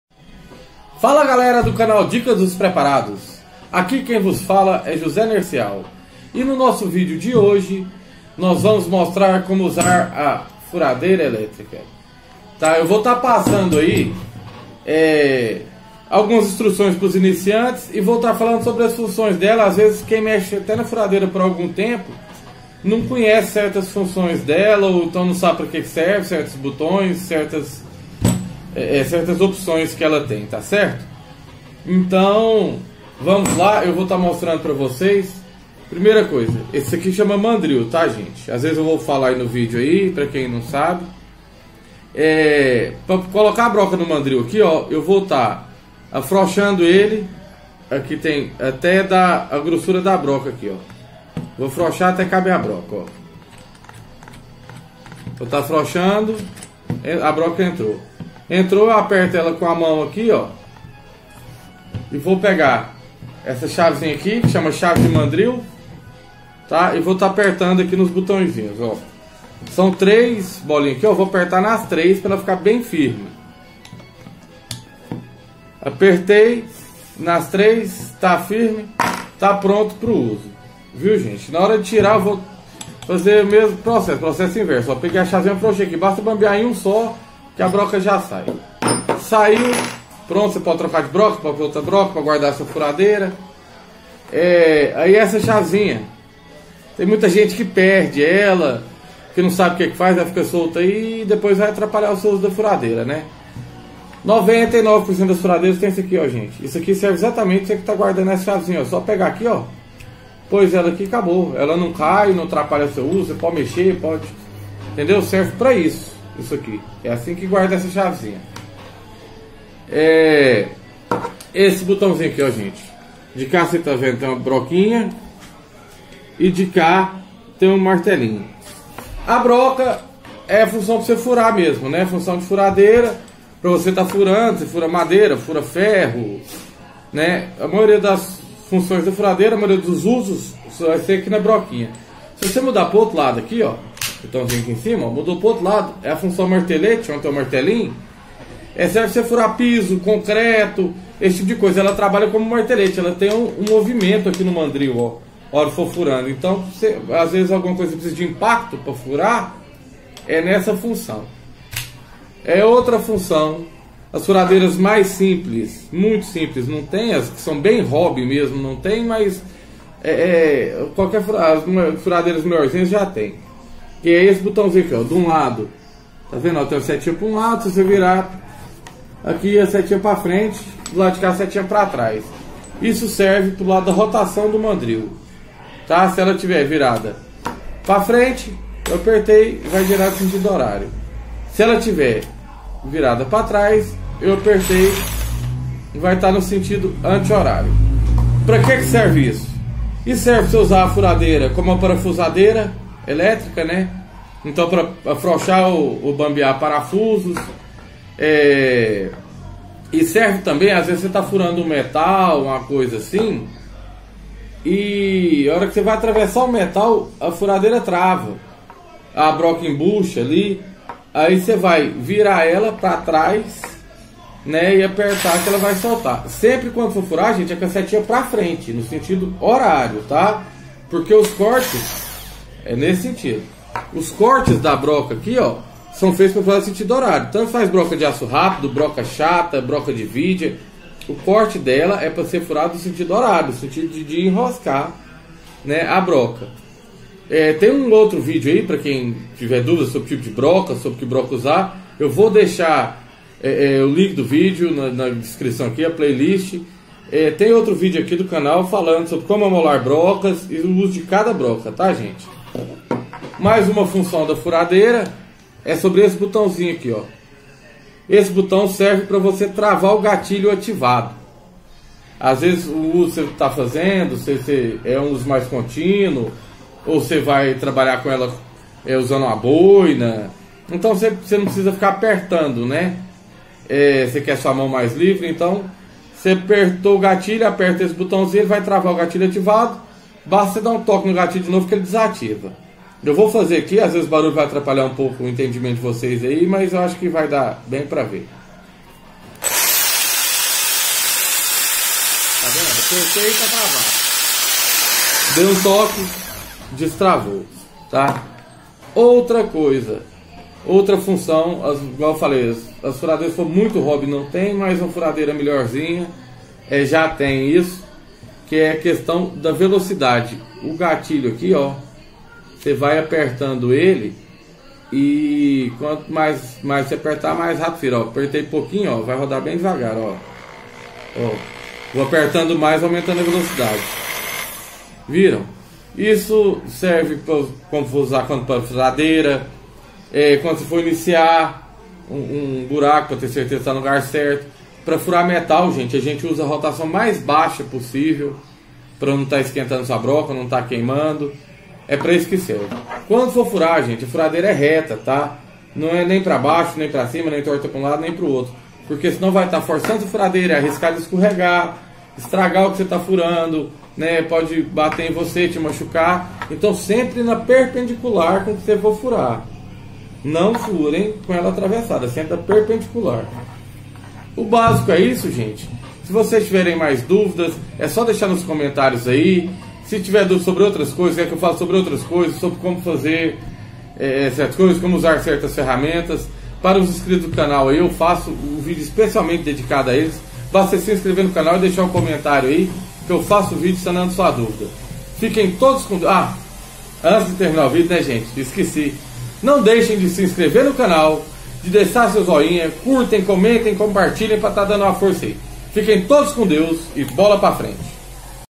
Como utilizar furadeira elétrica e suas funções
Explicação do uso da furadeira para iniciantes e funções dos botões e recursos.